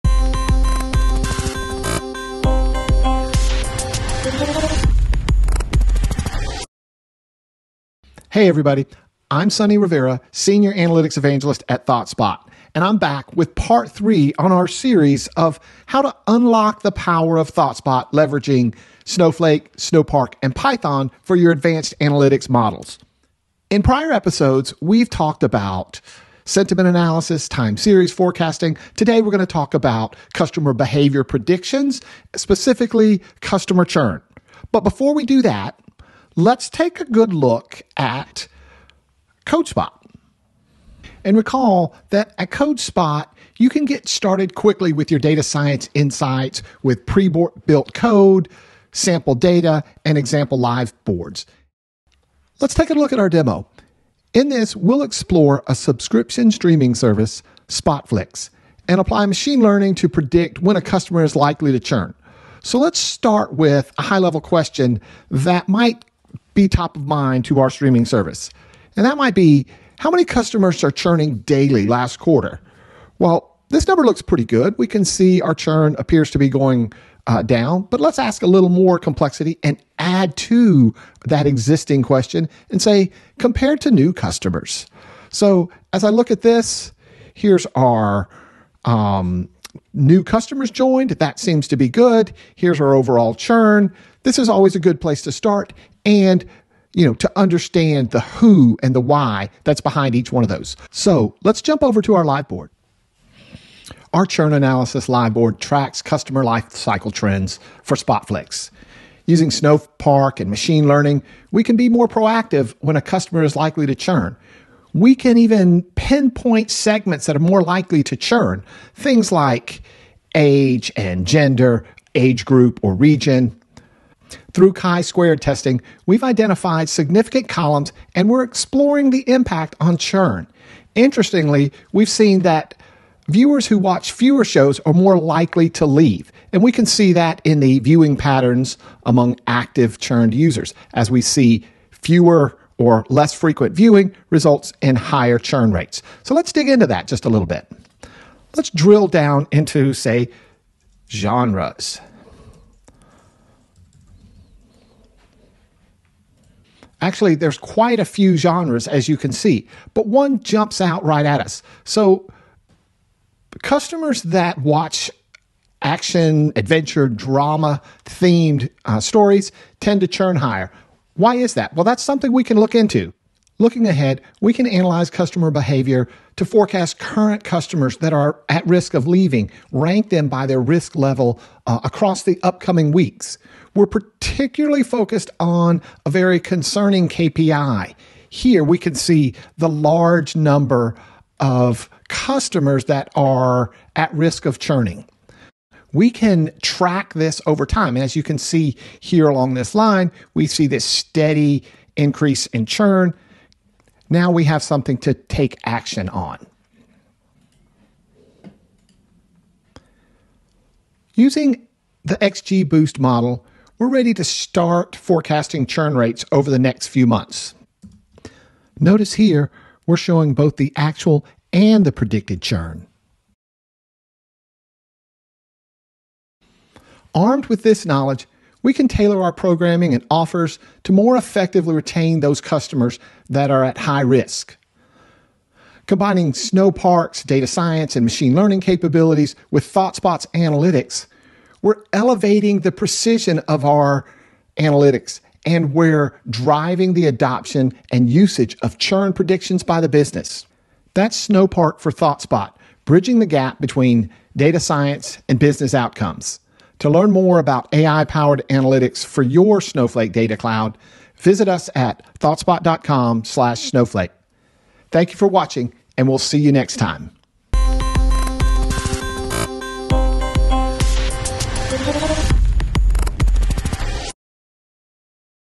ThoughtSpot for Snowpark: Predictive churn analysis
Use Snowflake, Snowpark Python, and machine learning in ThoughtSpot to uncover insights that guide strategic decisions.